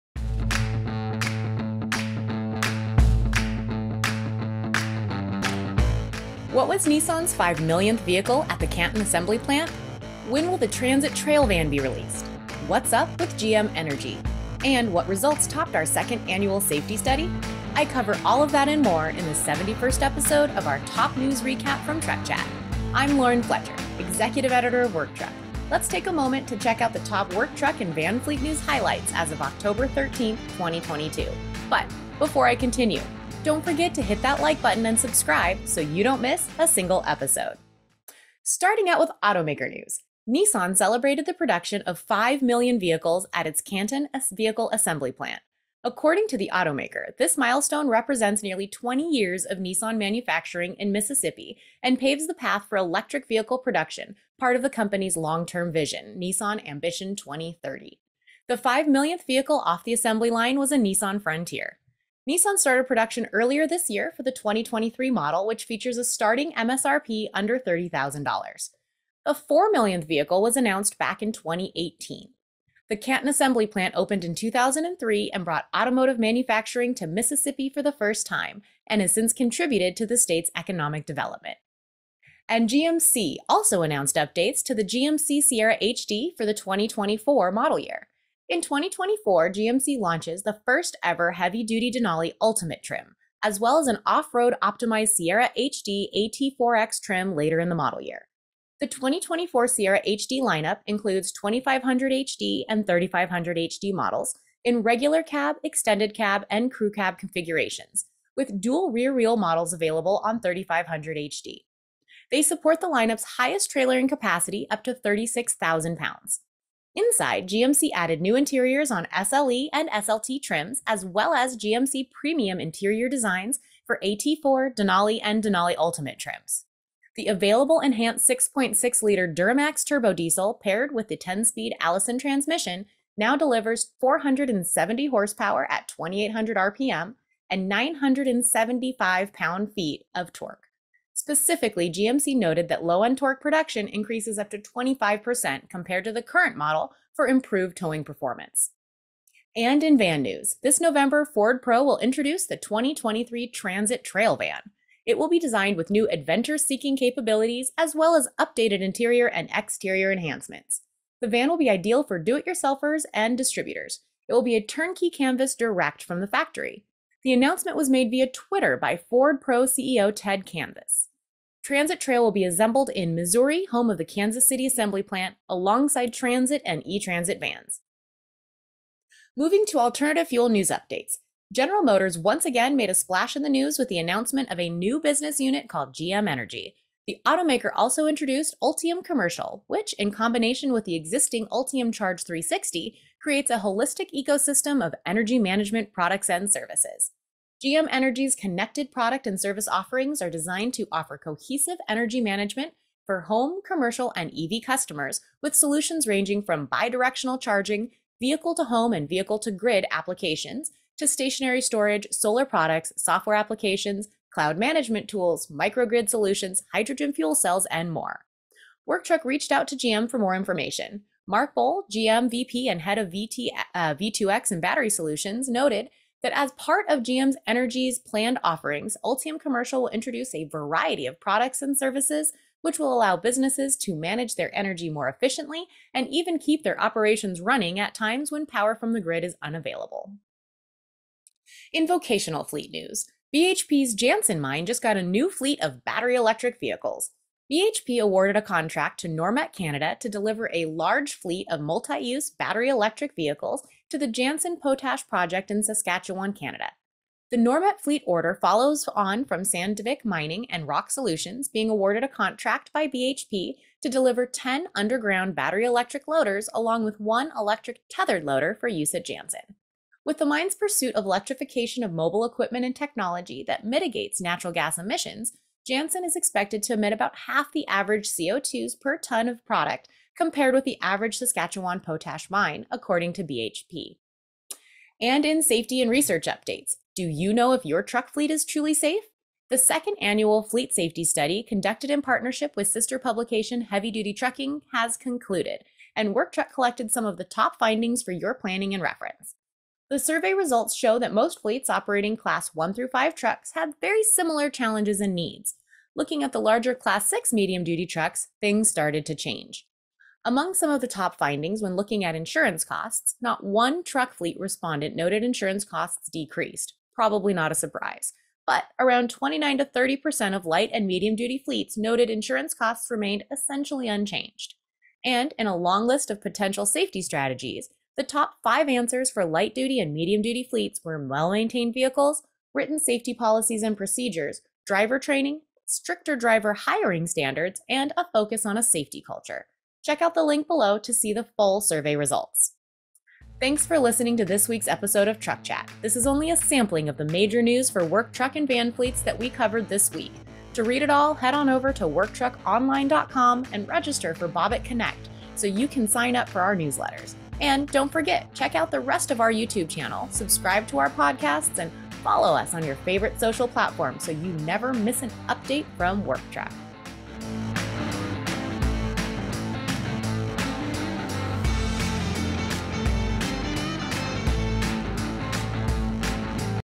Top Work Truck News for the Week of 10.13.22
This week in top Work Truck news, Lauren Fletcher, Executive Editor for Work Truck, shares the top worktruck news as of Oct. 13, 2022

🎙 Topics covered this week include:
- @Nissan produces its 5-millionth vehicle in Canton, Ohio
- @gmc Updates SierraHD for 2024
- @ford introducing 2023 Transit Trail van
- @GeneralMotors creates GM Energy business unit
- BHP Jansen mining gets battery-electric trucks (BEVs)

🚚 Visit Work Truck for all the latest industry news and information

🤝 Follow and connect with Work Truck on social media!

🎧 Prefer to listen? Check out the Work Truck podcast!

⏱ Timestamps ⏱
00:00 Introduction
00:40 Nissan Frontier is 5-millionth vehicle at Canton
01:22 GMC updates Sierra HD for 2024-MY
02:29 When will the Ford Transit Trail van be released?
03:01 What is GM Energy and Ultium Commercial?
04:01 Mining fleet gets battery-electric trucks
04:44 Work Truck's 2nd Annual Safety Study
05:43 Conclusion/Thank you